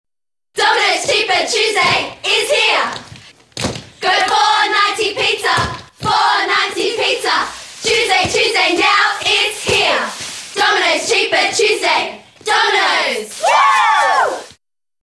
Domino's Cheaper Tuesday is HERE!!!!
WOOOOooooooHHHHHHHOOOOOO! Domino's Cheaper Tuesday is Here!! Only $4.90 pizza, $4.90 pizza Tuesday Tuesday now is here. Dominos Cheerelading advertising for Cheaper Tuesday - loving the cheerleaders, make me wanna buy pizza every Tuesday!!! Yeah, woooo hoooo!1